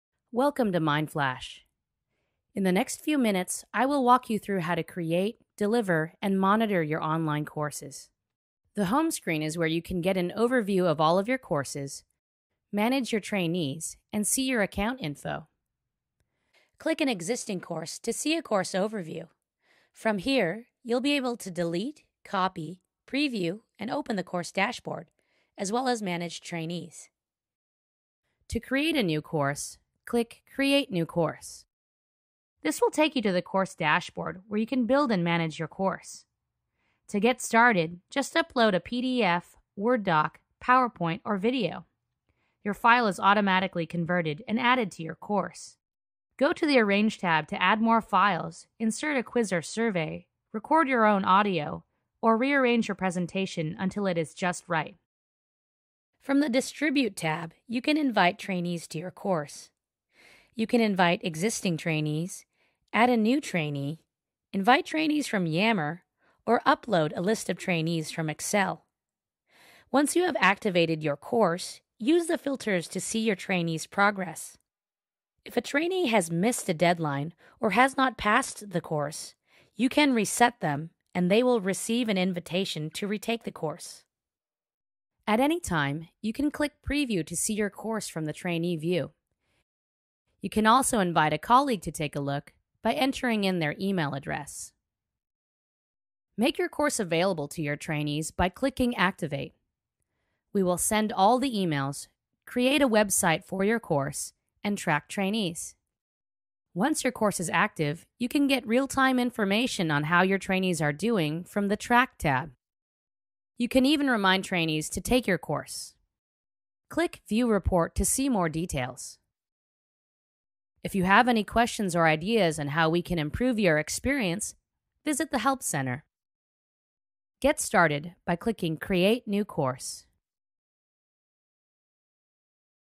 Mindflash Introduction (2016 update)
Overview of Mindflash online training platform. In 2 minutes the video will cover the features of the Mindflash Learning Management System (LMS).